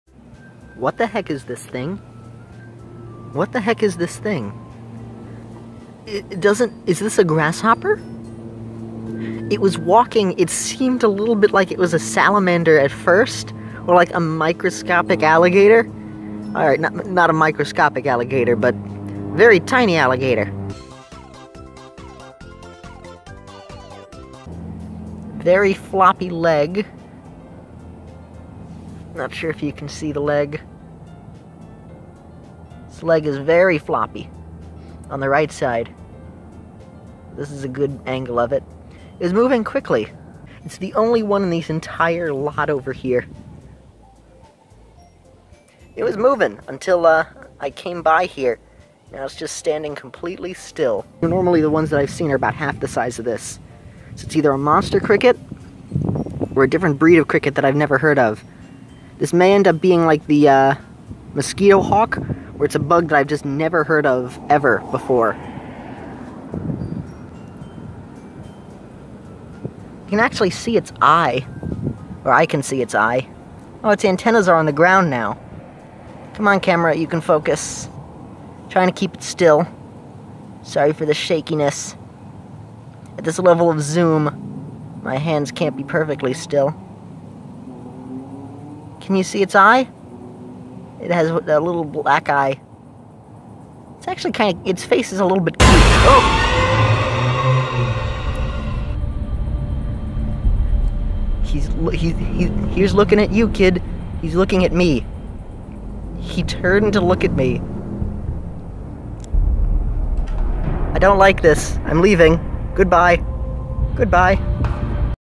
Giant Enemy Bug 2 ~ Electric Bugaloo
I apologize for the low quality of the video. I was scared of it and zoomed in so I wasn't too close. This was when I was in Florida and I thought it was a lizard at first, so I was freaked out to see that it wasn't.
This video was not too interesting, so I made it an experiment of sorts regarding music. I wanted to make the best out of this for editing practice.